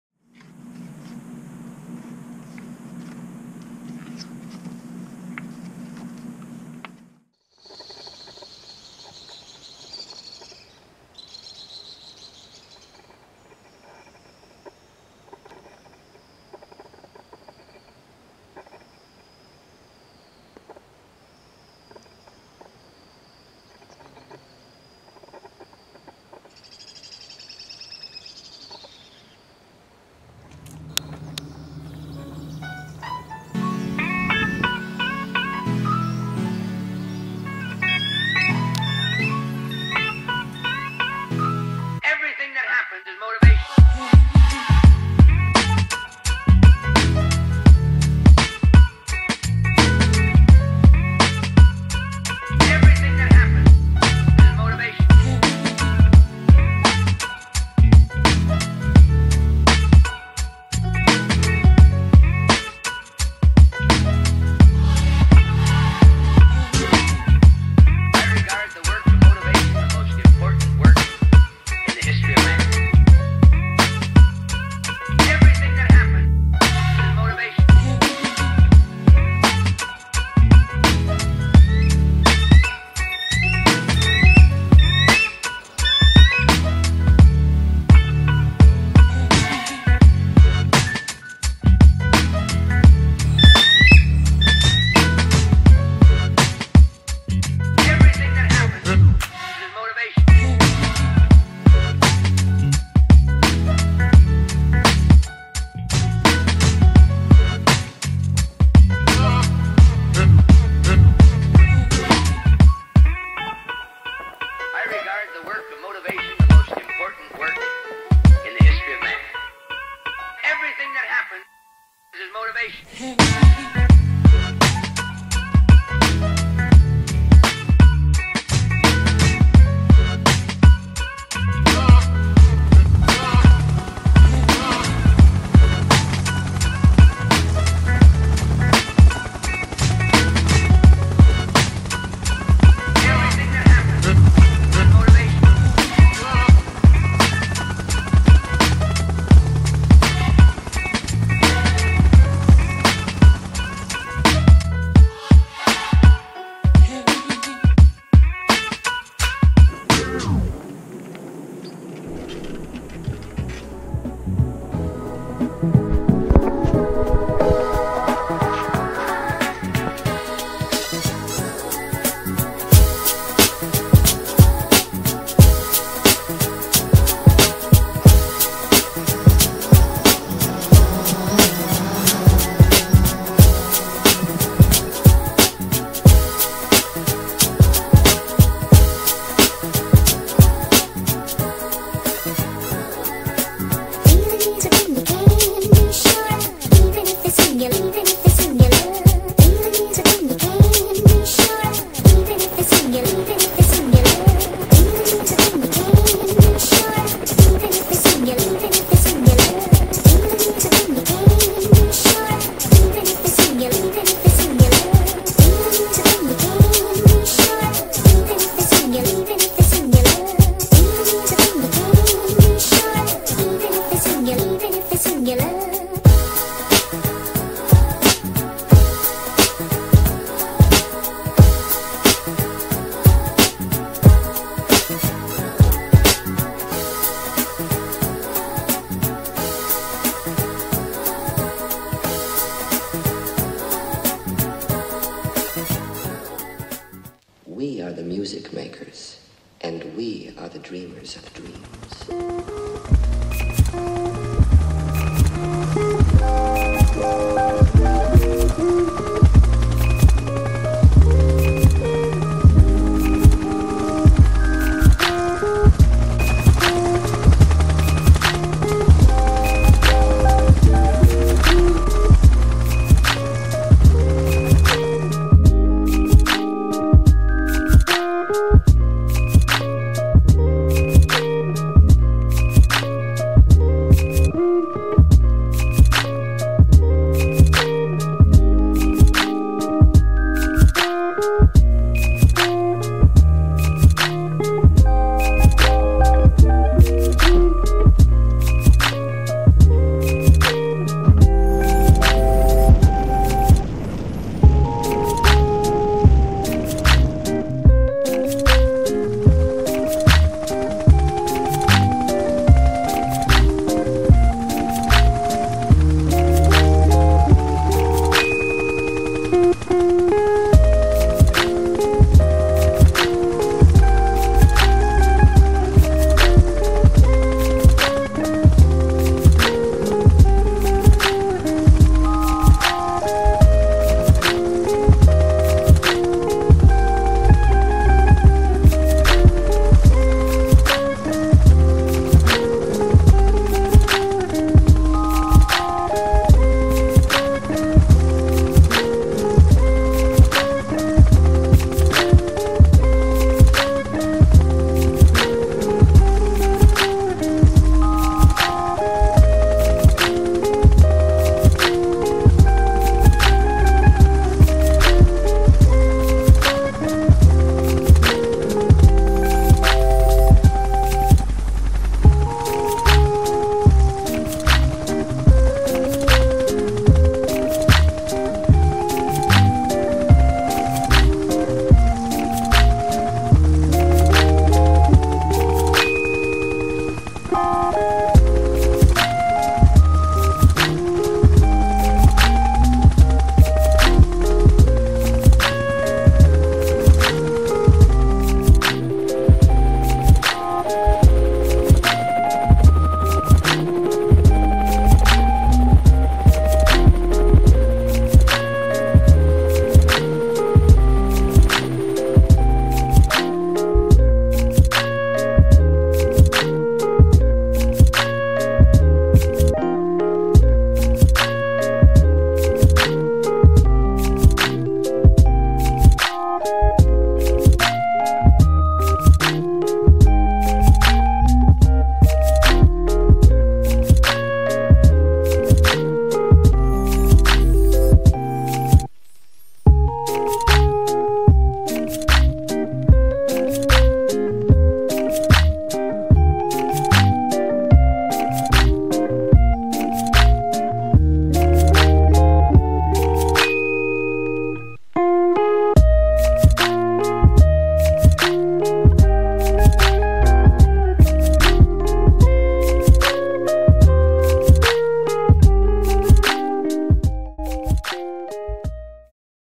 Painted Canyon, Mecca CA by Drone (DJI Phantom 4)
Video from Mecca CA, hiked up to the top of Painted Canyon.